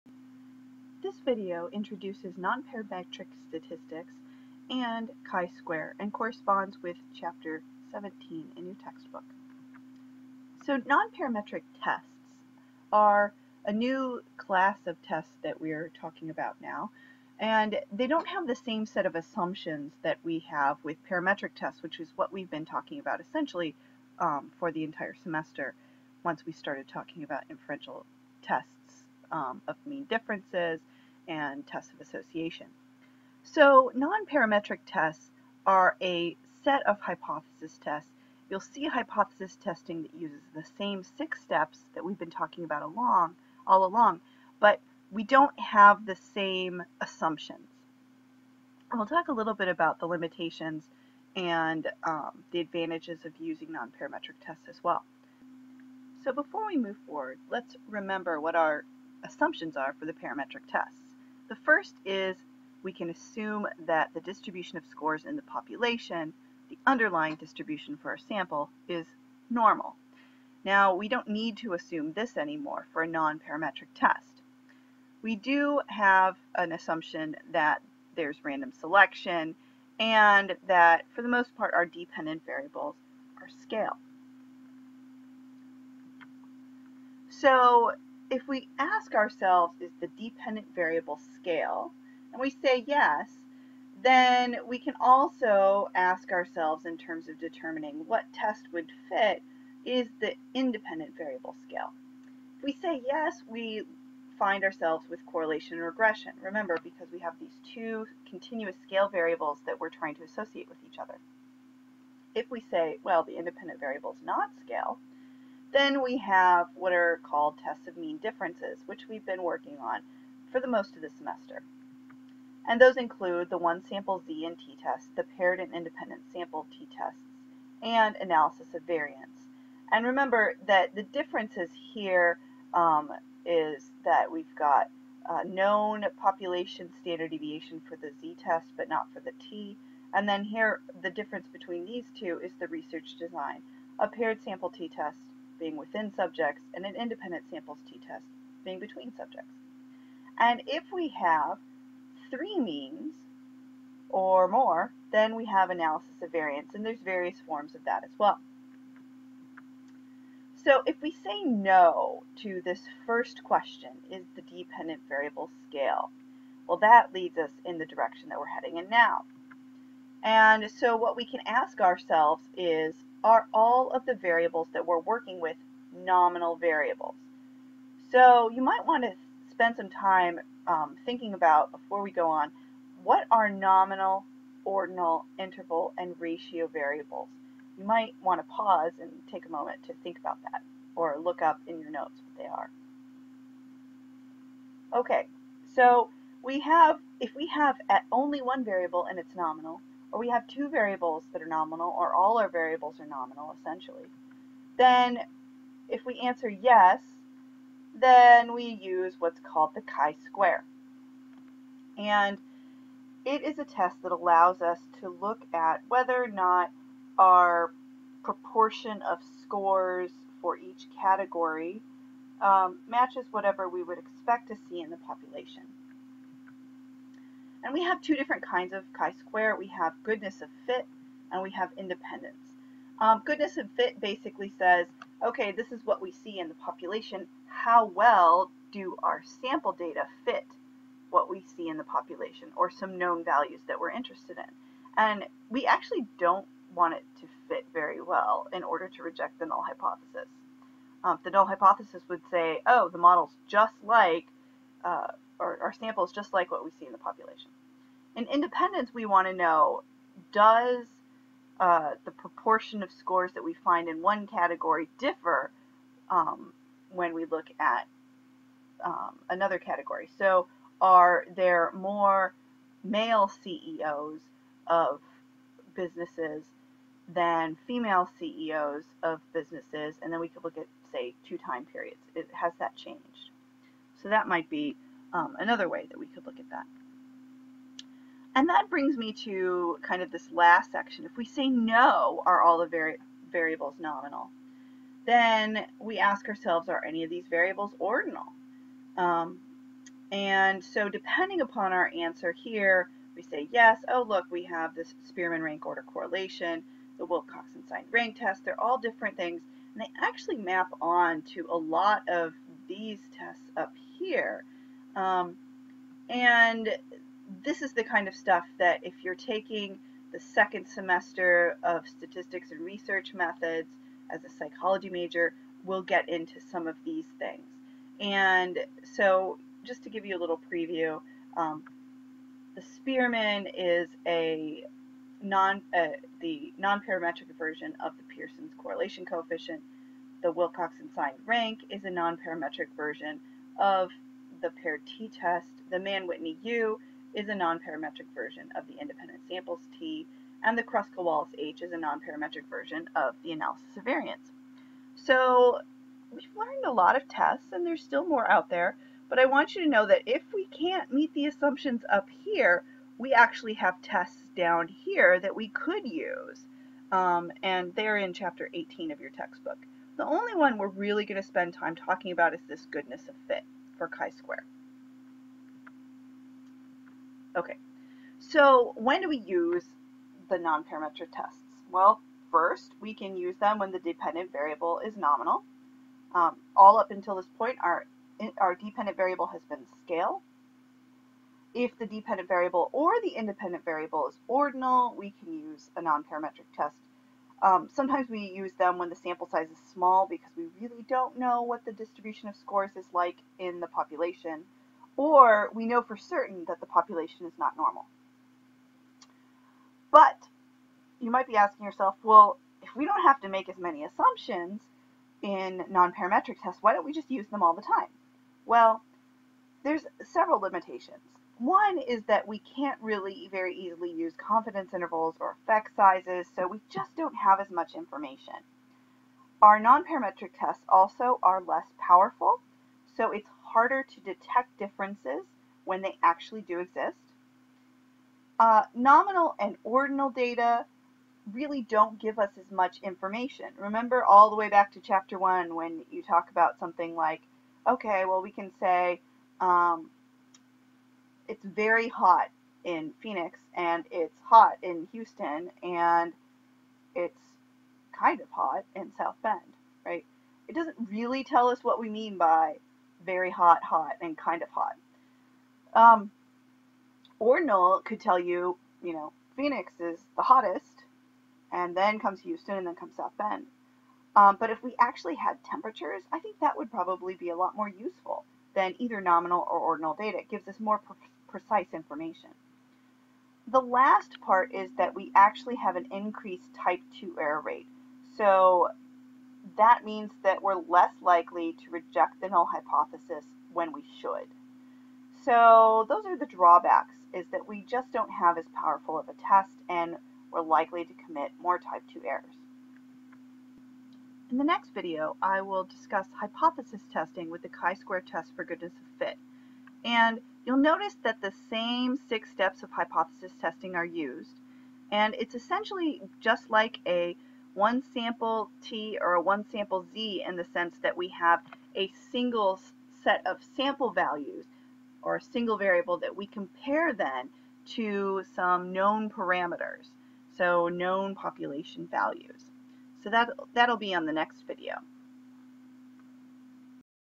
Introducing Chi-Square
Introduces nonparametric tests and the Chi-square test.